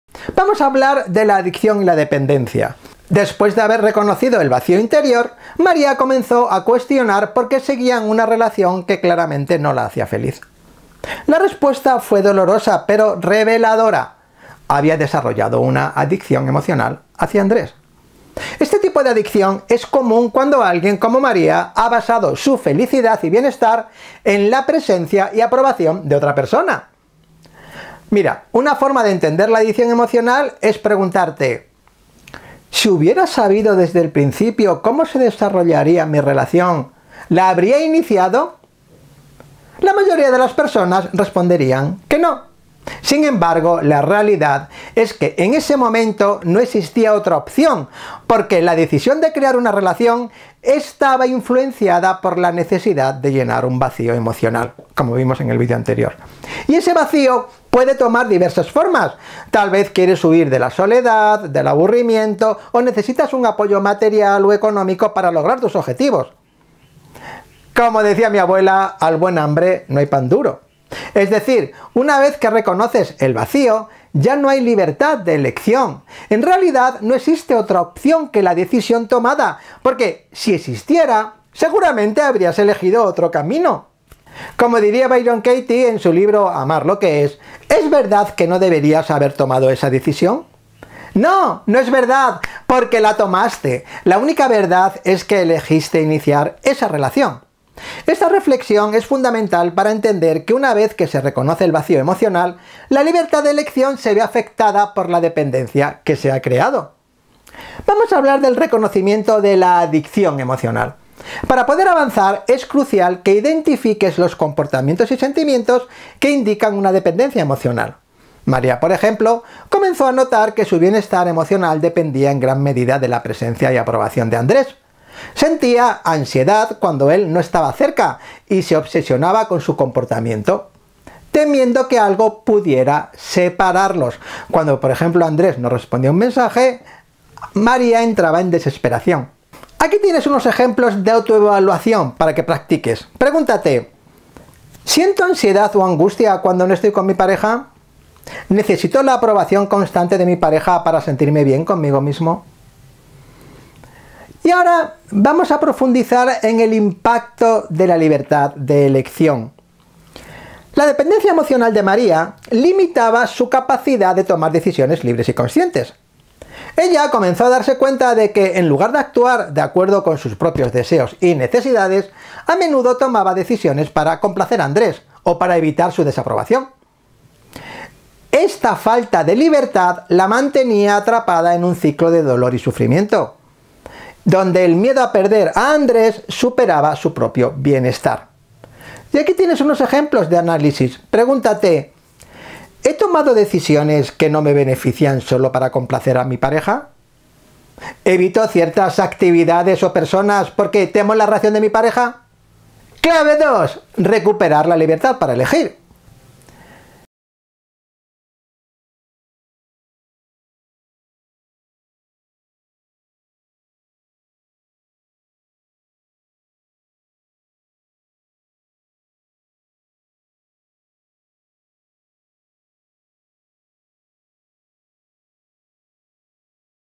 ¿Por Qué es Tan Difícil Dejar una Relación? Aprende a Superar la Dependencia Emocional
Descubre cómo liberarte de la dependencia emocional en tu relación.

También obtendrás acceso a contenido exclusivo sobre mi mentoría Tu Pareja Consciente Mentoring.

En este video, profundizamos en por qué es tan difícil dejar una relación que no te hace feliz. A través de los conceptos de Un Curso de Milagros, te guiaré paso a paso para reconocer la adicción emocional, cómo afecta tu capacidad de tomar decisiones y qué puedes hacer para recuperar tu autonomía emocional. También te enseñaré a tomar decisiones conscientes desde un lugar de claridad y libertad interior, en lugar de hacerlo desde la necesidad o el miedo.

Aprenderás:
- Cómo identificar la dependencia emocional en tu relación.
- Estrategias para desarrollar tu autonomía emocional y tomar decisiones libres.
- Consejos para tomar decisiones desde la claridad y no desde la necesidad o el miedo.

¿Listo para transformar tu relación?
Mira el video completo y descubre cómo puedes aplicar el Sistema RICo para superar tus desafíos de pareja y vivir relaciones conscientes.